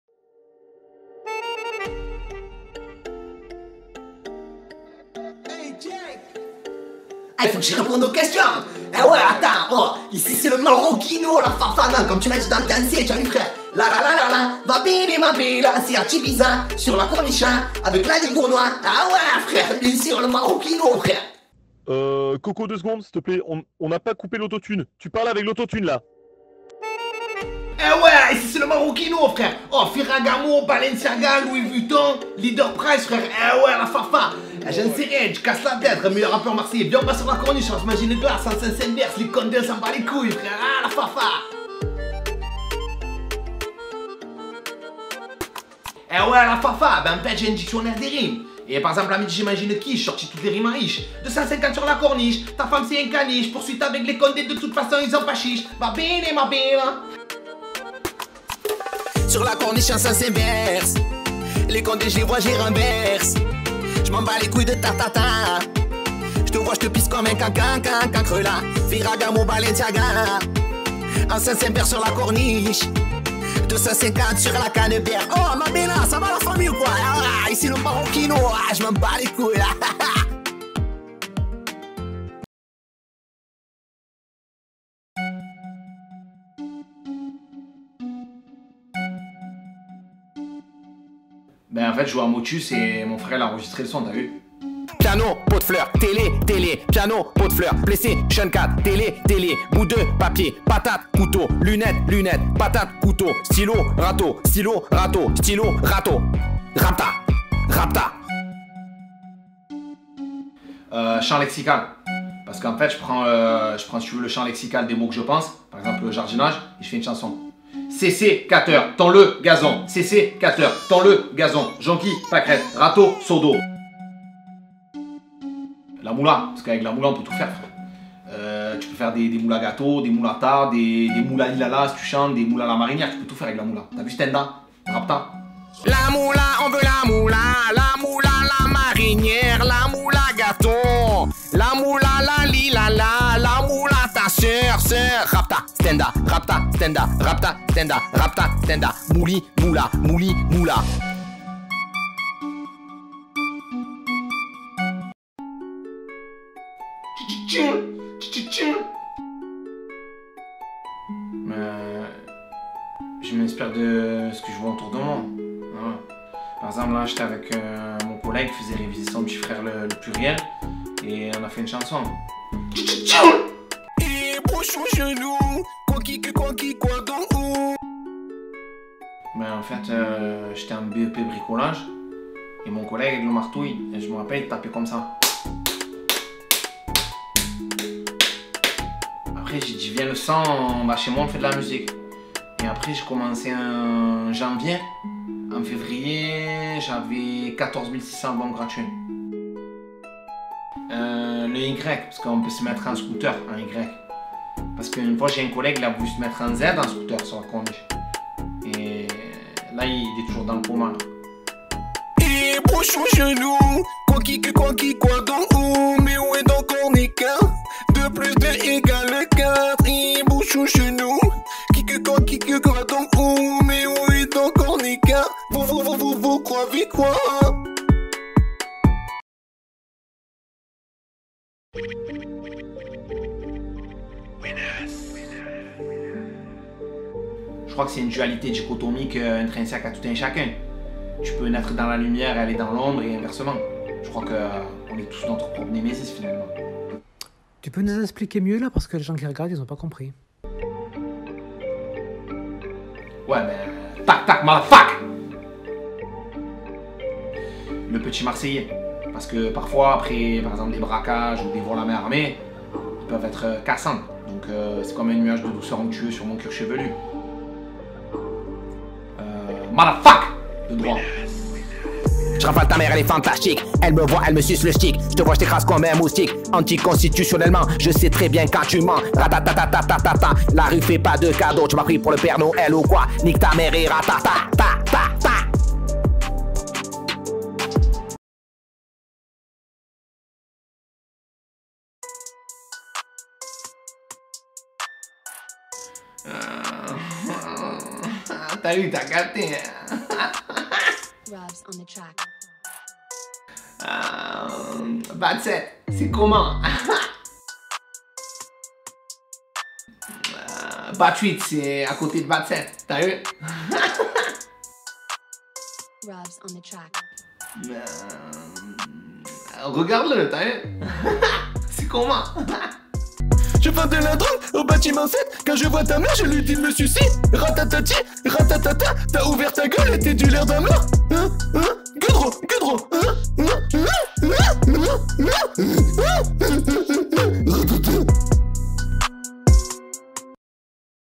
Le MÉGA BATTLE des interviews de Rap Konbini!!
La Team il est temps de voter en commentaire pour votre rapper préféré et il aura son clip!
Perso je vote pour Le Marocchino 😂😂

Crédits vidéo Patryck lebrun